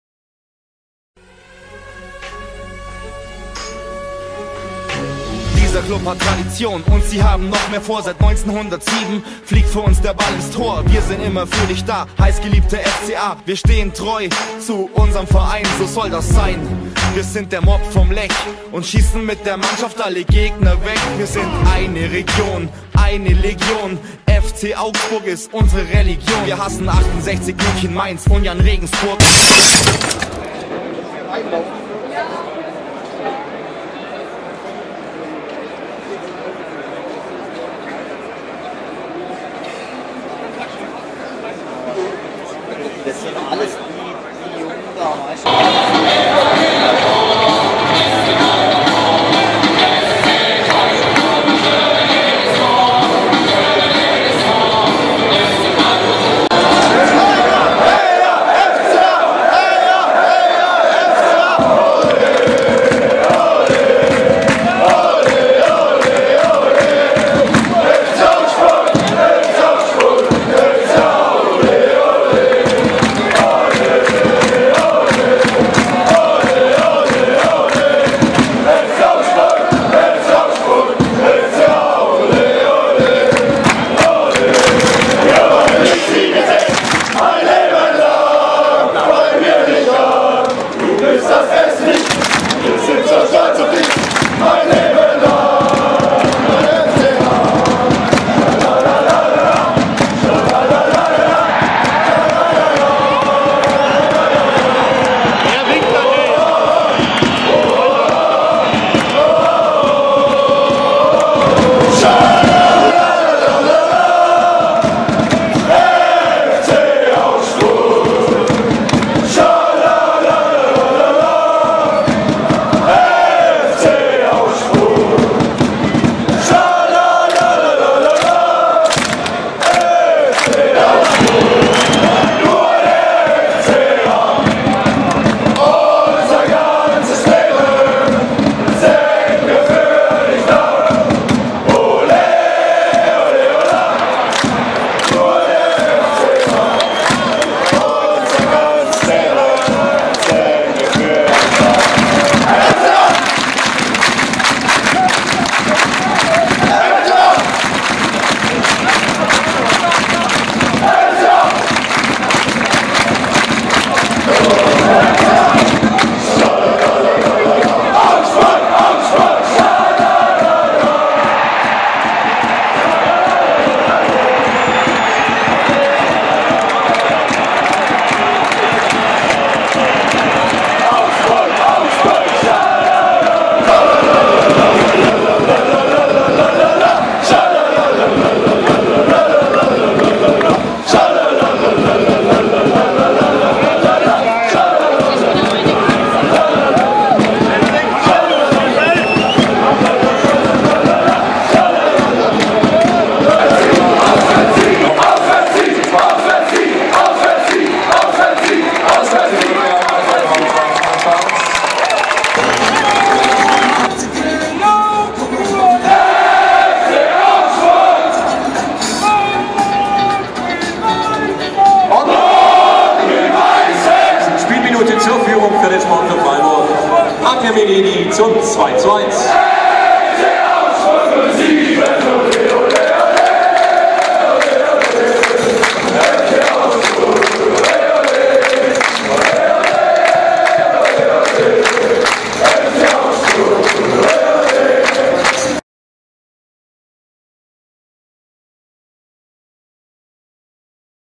SC Freiburg vs. FC Augsburg | 22.2.2014 | 22. Bundesliga-Spieltag 2013/14
SC Freiburg vs. FC Augsburg 1907
22. Bundesligaspieltag 2013 / 14
im Dreisamstadion Freiburg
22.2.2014

Endergebnis: 2:4
Tore FCA: Werner, Verhaegh, Altintop, Hahn

Copyright by SCHWABENREPORT 1907

(Dieses Video ist urheberrechtlich geschützt! Anderweitige Verwendung wie Weitergabe, Verkauf, Verleih und Ausstrahlung ist untersagt und bedarf meiner Zustimmung!)